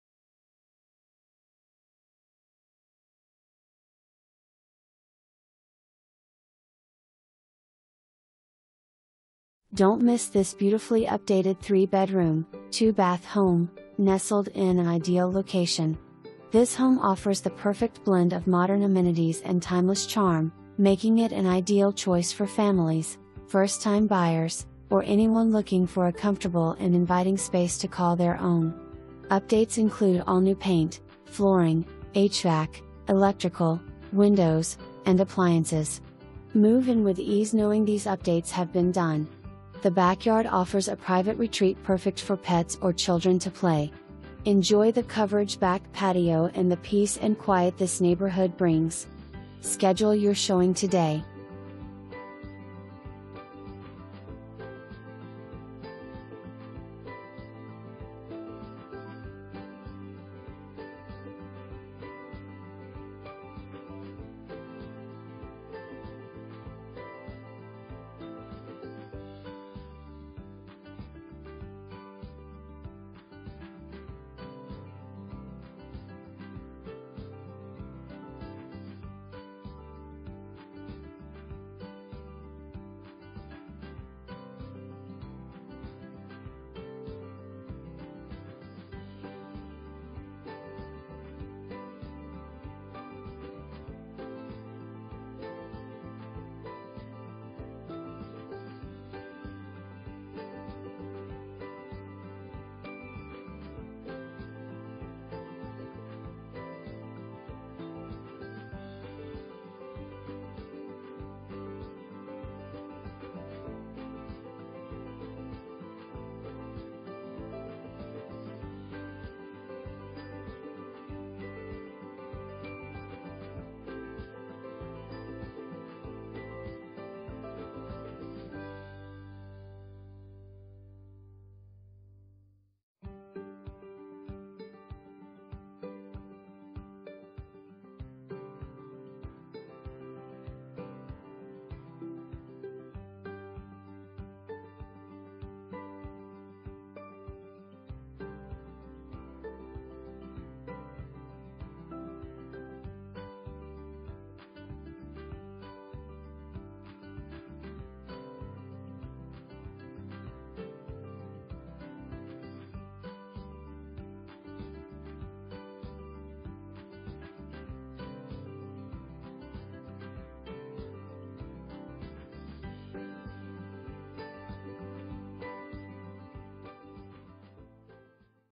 Homes for Sale - 3300 Greg, Tyler, TX
Don't miss this beautifully updated 3-bedroom, 2-bath home, nestled in an ideal location. This home offers the perfect blend of modern amenities and timeless charm, making it an ideal choice for families, first-time buyers, or anyone looking for a comfortable and inviting space to call their own. Updates include all new paint, flooring, HVAC, electrical, windows, and appliances. Move in with ease knowing these updates have been done. The backyard offers a private retreat perfect for pets or children to play. Enjoy the coverage back patio and the peace and quiet this neighborhood brings. Schedule your showing today!

2 full bath

Judy Kunzman
Keller Williams